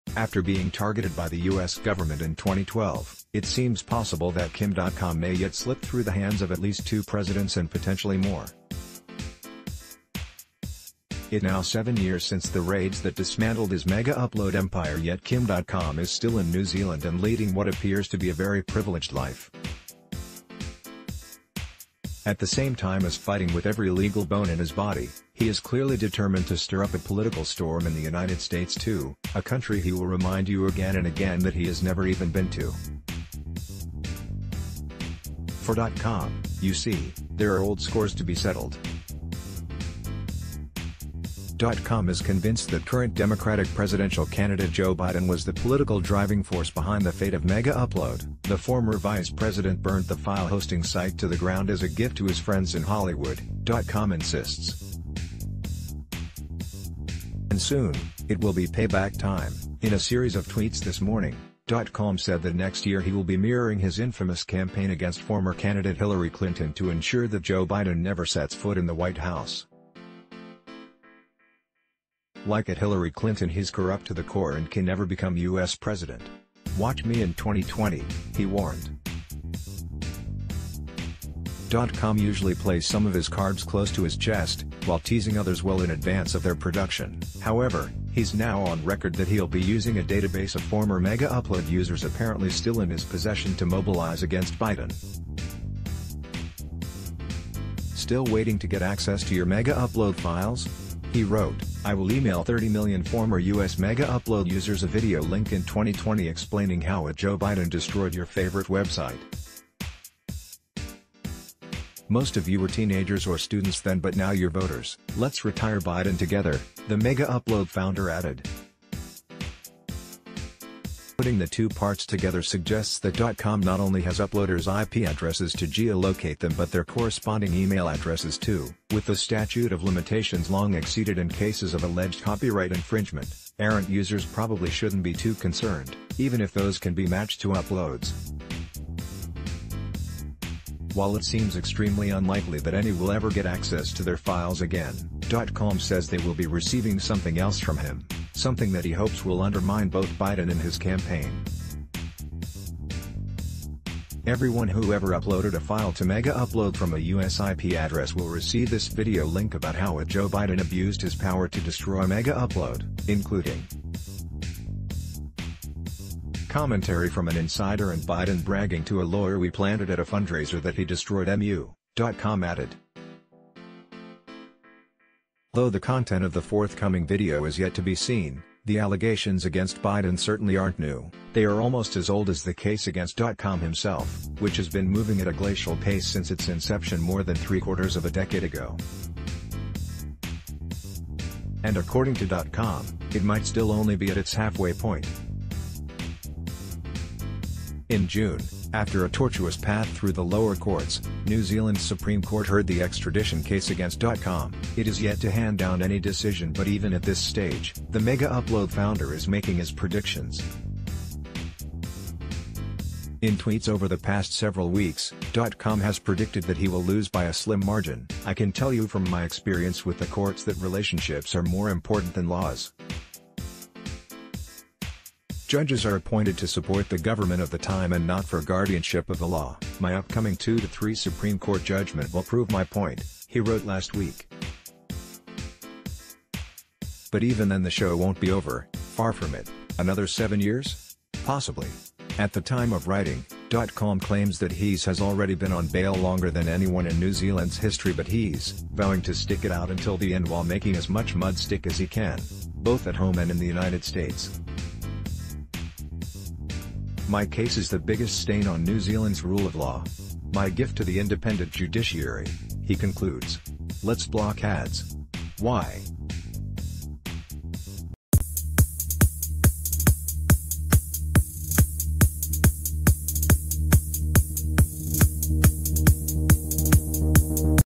Kim Dotcom Set to Mobilize Former Megaupload Users Against Joe Biden - TorrentFreak | Politics Ne...
Kim Dotcom Set to Mobilize Former Megaupload Users Against Joe Biden - TorrentFreak | Politics News Network

After being targeted by the US government in 2012, it seems possible that Kim Dotcom may yet slip through the hands of at least two presidents and pot...